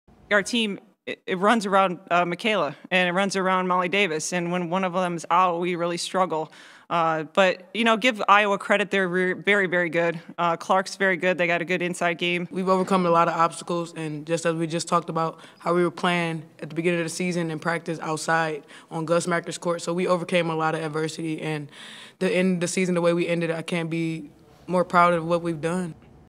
CMU falls to Iowa in NCAA Tournament first round
Micaela Kelly and Heather Oesterle react following Central Michigan's loss to Iowa in the first round of the NCAA Tournament.